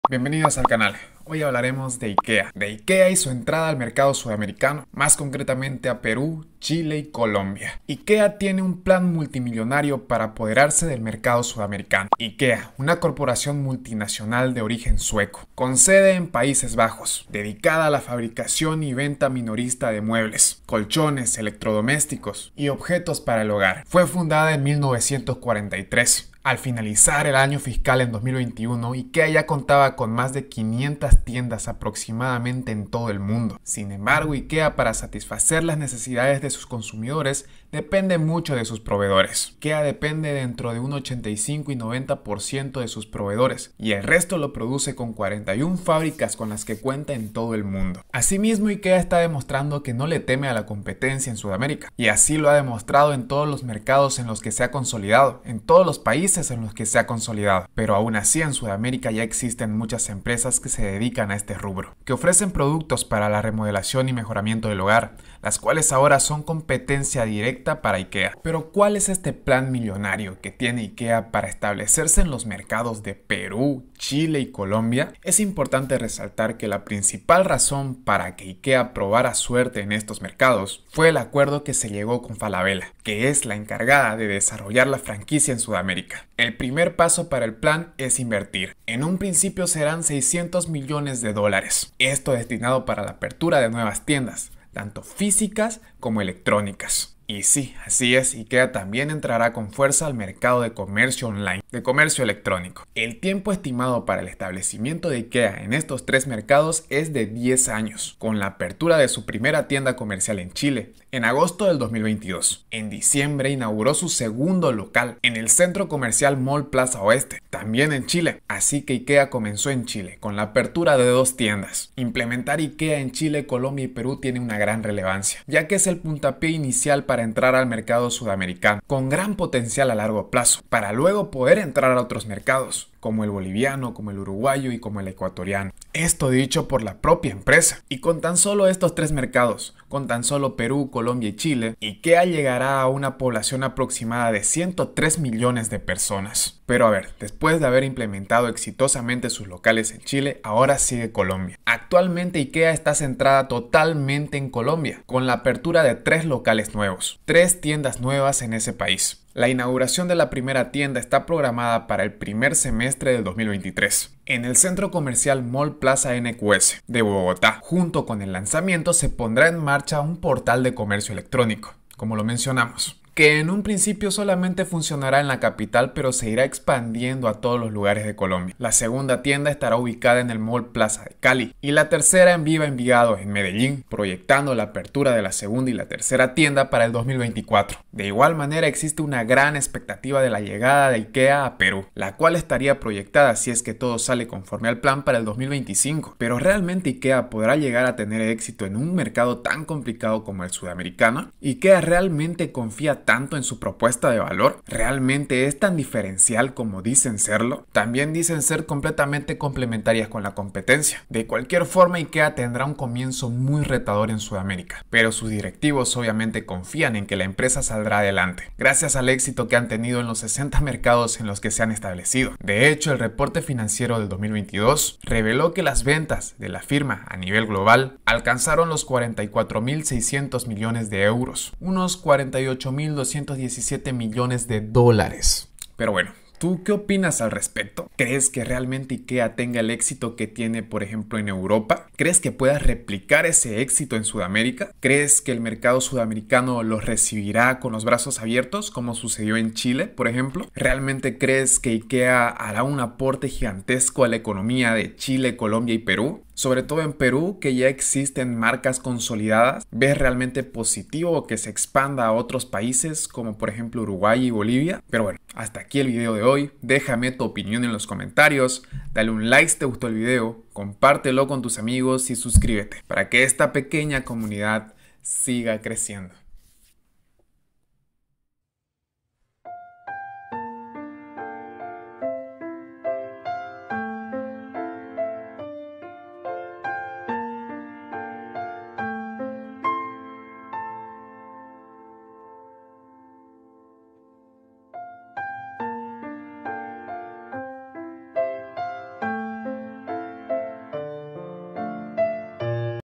Ikea en Perú, Chile y Colombia | Podrá Ikea dominar el mercado Sudamericano ??
IKEA en Perú Chile y Colombia. El plan millonario de Ikea, para apoderarse del mercado sudamericano,